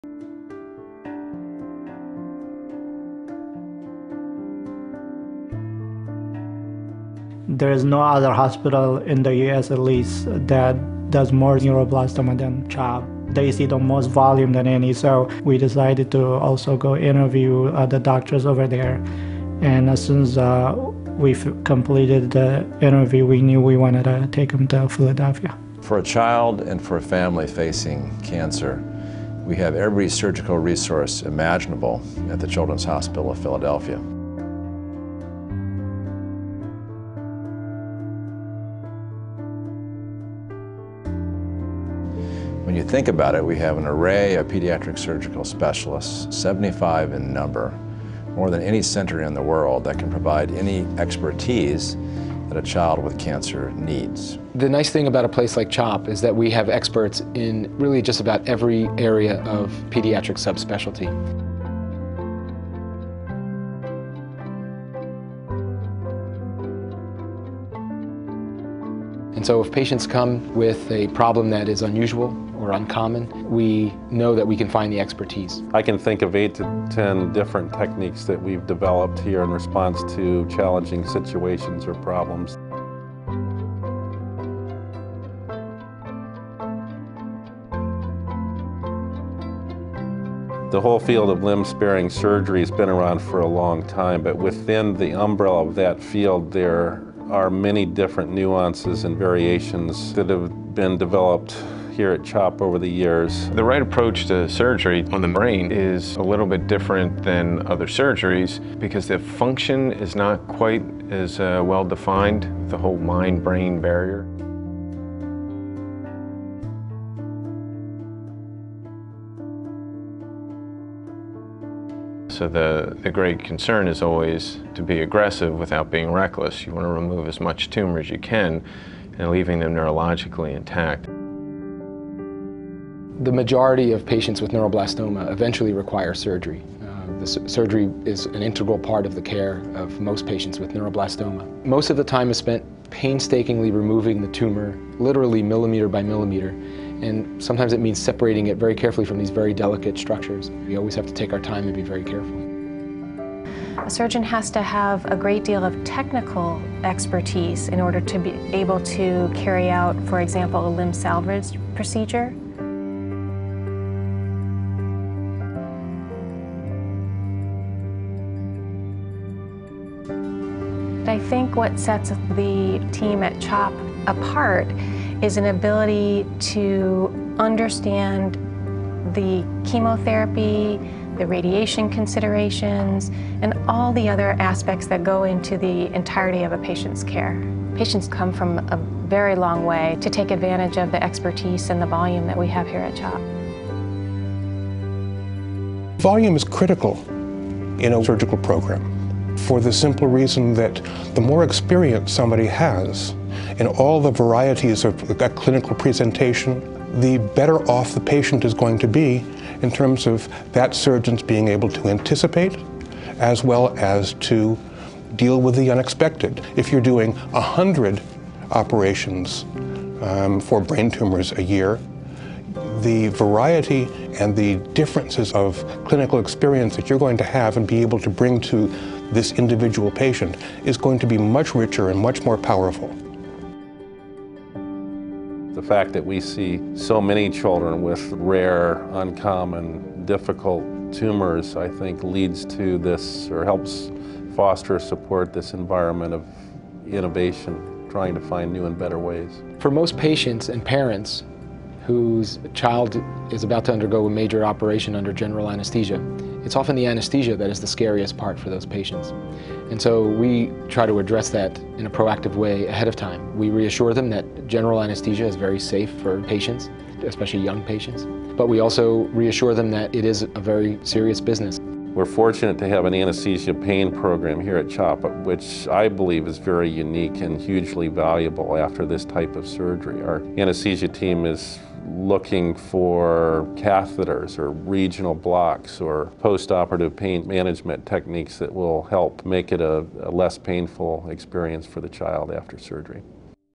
Surgery for Children with Solid Tumors - The Children's Hospital of Philadelphia
The Children's Hospital of Philadelphia is staffed with 75 pediatric surgical specialists, and experts representing every pediatric subspecialty. The surgical oncology program at CHOP has access to every surgical resource imaginable, and vast experience operating on the most rare and unusual pediatric cancers, as well as neuroblastoma, brain tumors and sarcomas. Several pediatric surgical techniques were developed at The Children's Hospital of Philadelphia, and the environment of innovation at the Hospital drives our surgeons to keep finding new and better ways to operate on pediatric cancers.